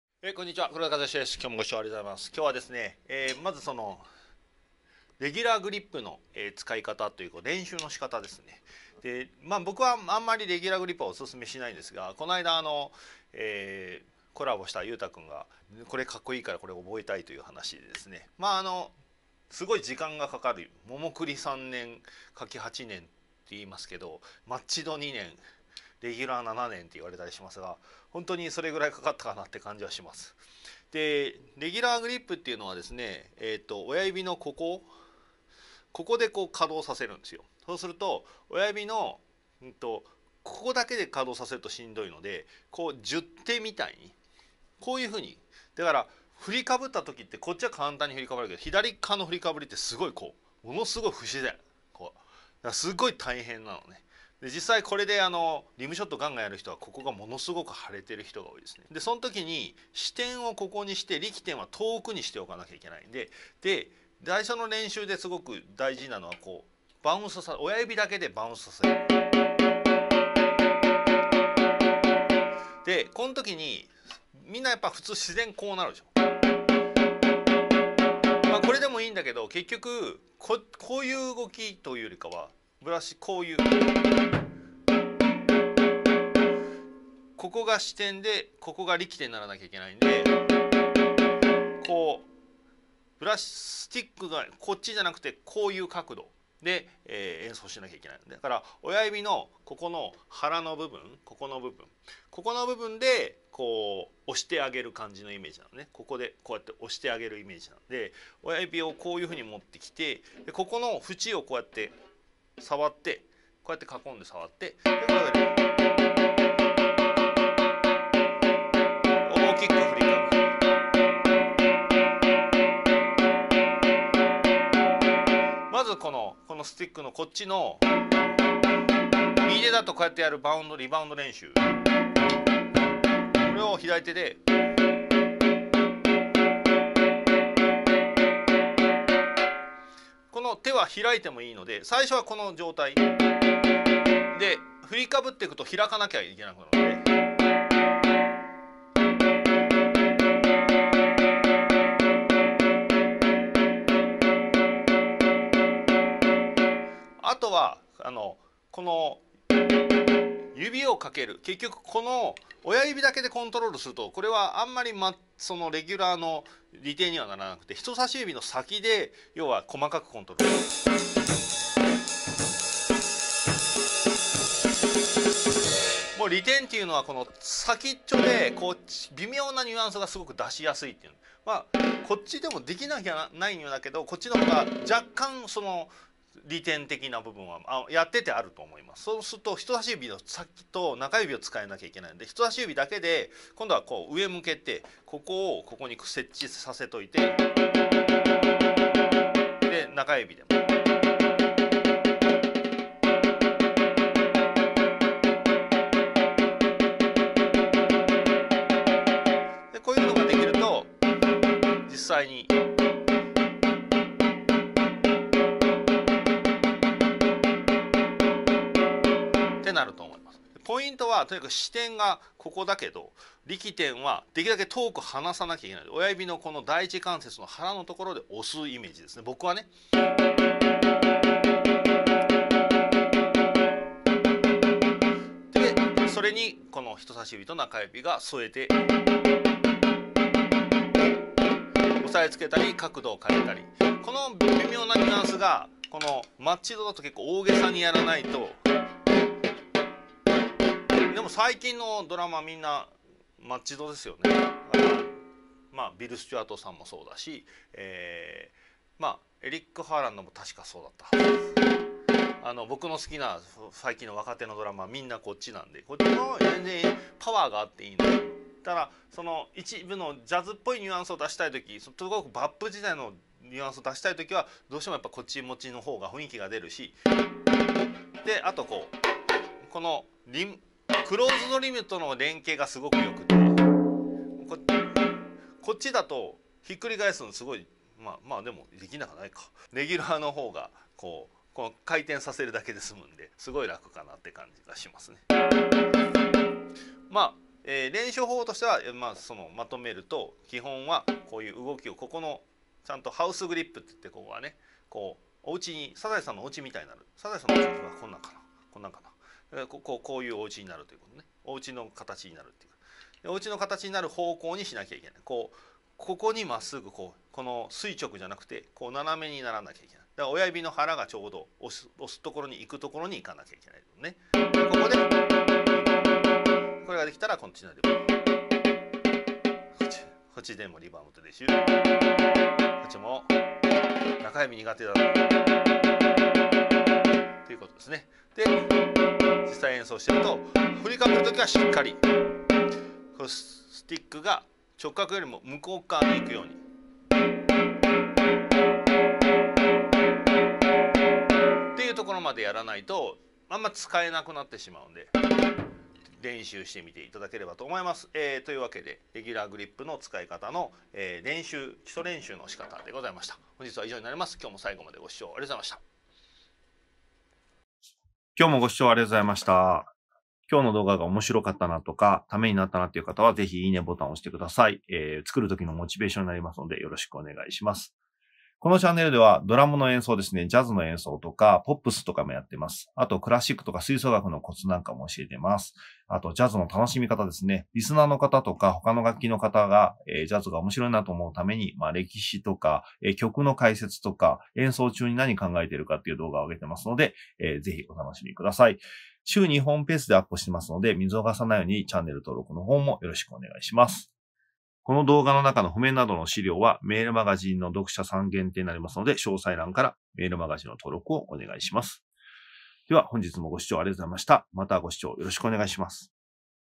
レギュラーグリップ基礎練習 How to practice for regular grip?
レギュラーグリップの基礎練習
How to practice for regular grip?

レギュラーグリップって
どうやって練習すればいいですか？
という質問いただいたので
つくりました。

レギュラーって
最近の若手はあんまりいないんですよね
ビルスチュワートもエリックハーランドも
マッチドです

おそらく現代のジャズでは
演奏のパワーを求められるから
の気がします

とはいえ、繊細なフレーズの曲は
やっぱりレギュラーが向いています
なにより見た目がかっこいいです！

というわけで、ストロークの練習方法を
動画でまとめてみました！

【参考動画】
バディリッチ（レギュラー）

エリックハーランド（マッチド）

ビルスチュワート（マッチド）